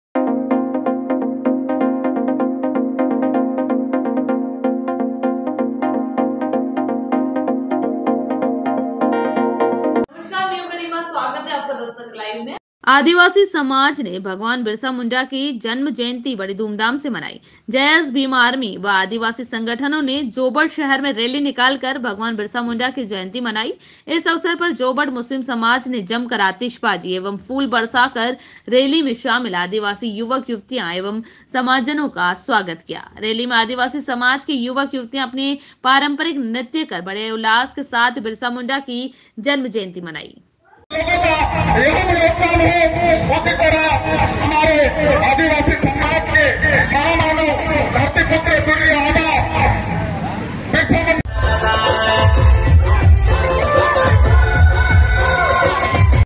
आदिवासी समाज ने भगवान बिरसा मुंडा की जन्म जयंती बड़ी धूमधाम से मनाई
@dastaklivenewschannel @news@jobat@जयस भीमआर्मी व आदिवासी संगठनों ने जोबट शहर में  रैली निकाली